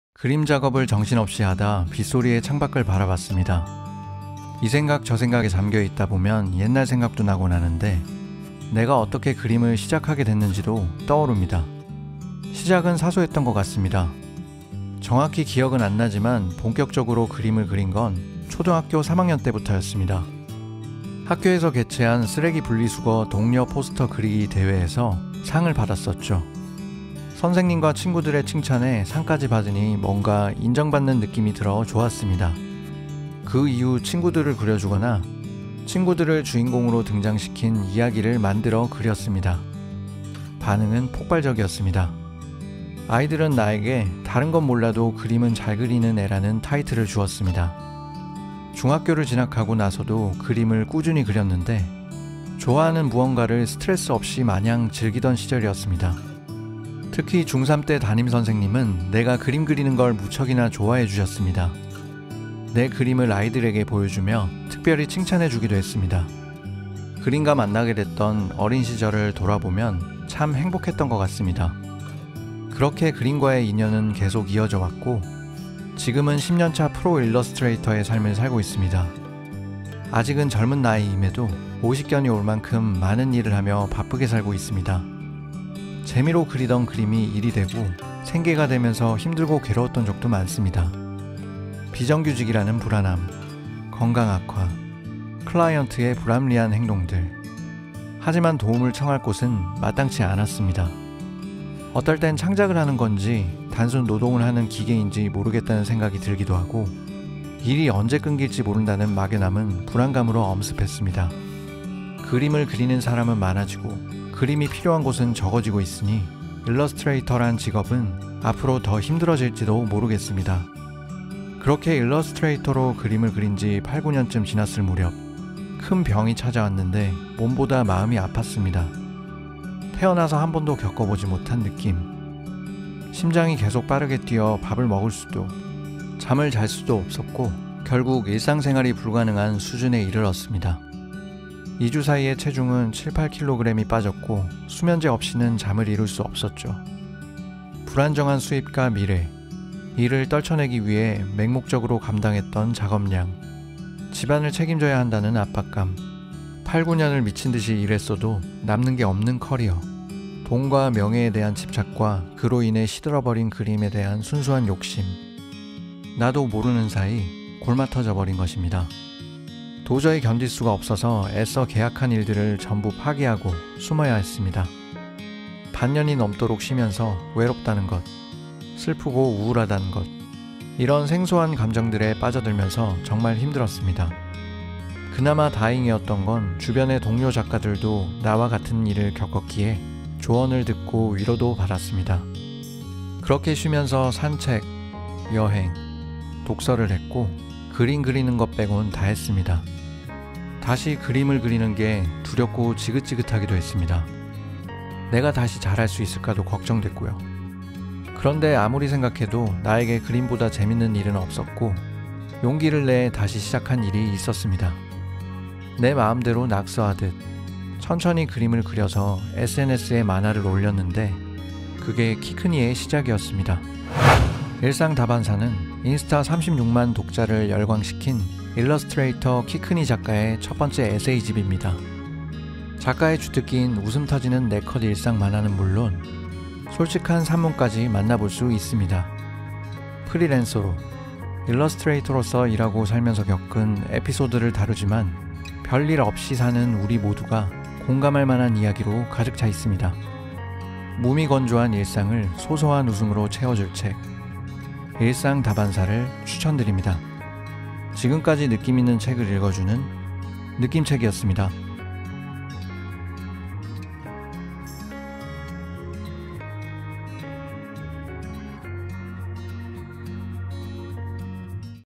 [이벤트종료] 일상, 다 반사 l 키크니, 에세이 l 느낌책, 구독자이벤트
[느낌책 책 증정 구독자이벤트]
채널을 구독하시고 댓글로 이 영상의  감상평을 적어주신 분 중
 총 3분을 추첨하여 이 책을 보내드립니다
당첨자는 10월 28일 다음 콘텐츠의 업로드와 함께
이 영상의 고정 댓글로 발표됩니다:)

인스타 36만 팔로워를 사로잡은 작가 키크니의 첫 에세이!
작가 키크니의 자전적 이야기와
프리랜서들 혹은 평범한 우리의 일상과는 멀지 않은
공감 이야기로 가득한 책 [일상, 다 반사]를 추천드립니다

이 컨텐츠는  출판사 샘터 의 지원을 받아 제작 되었습니다.

대본&나레이션
S

편집
조커스마일